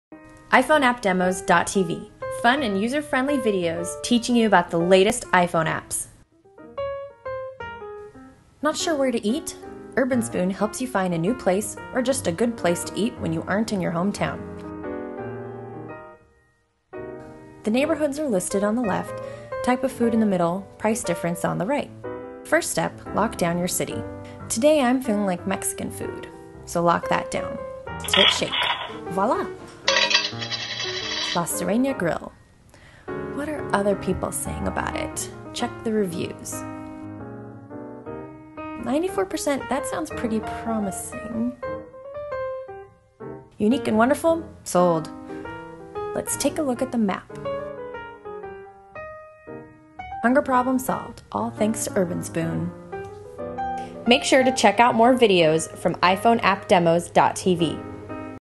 Urbanspoon app - iPhone App Demo by iPhoneAppDemos.tv
Whether you're in a new city, or just want to try something new, Urbanspoon can help you decide on a new restaurant to try out.